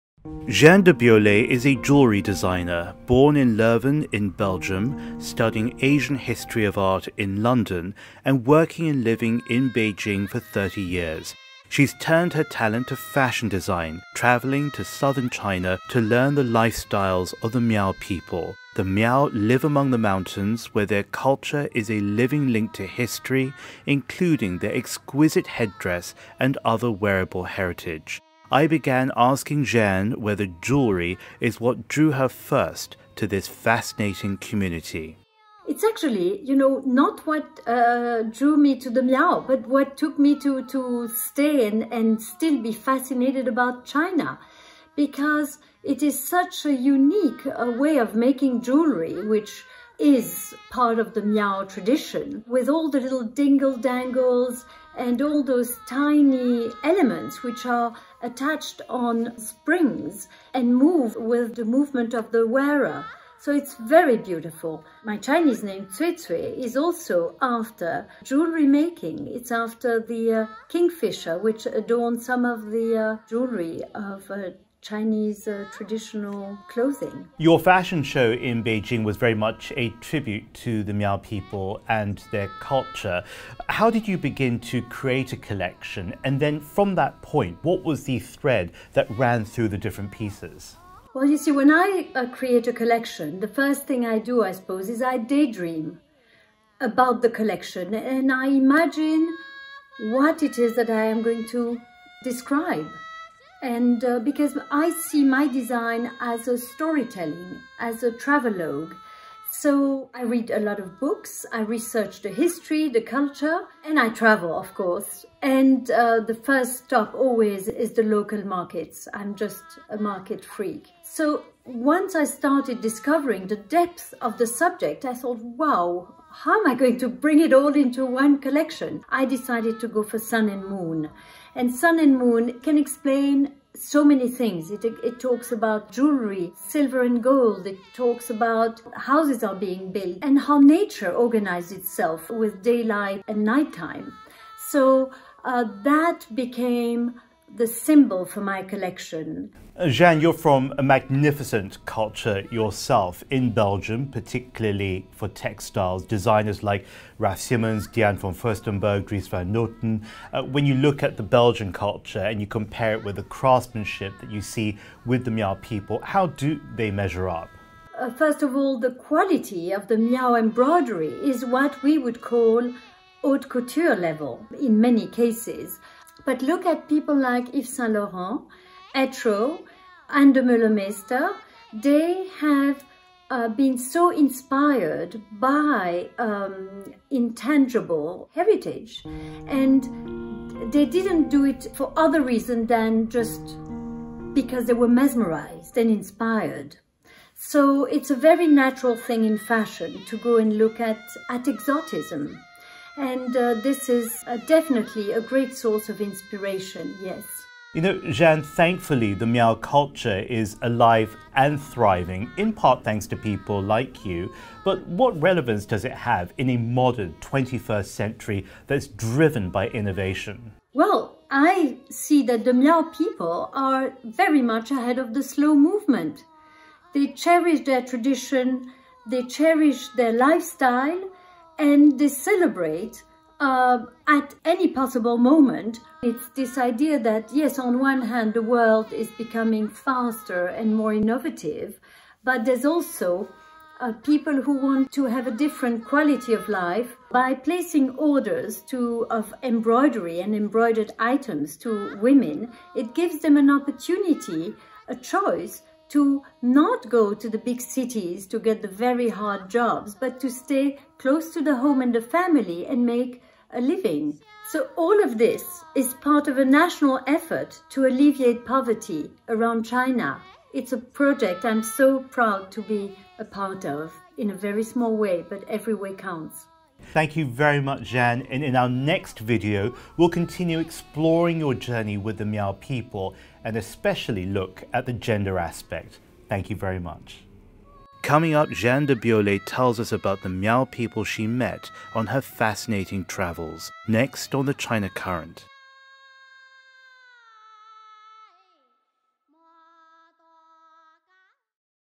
珠寶和苗族 | The China Current 粵語
Jehanne de Biolley是一位出生於比利時魯汶的珠寶設計師，在北京工作和生活了30年。她來到中國南方學習苗族人的生活方式。

大家可以在Facebook 、 Twitter 及YouTube  找到我們的視頻!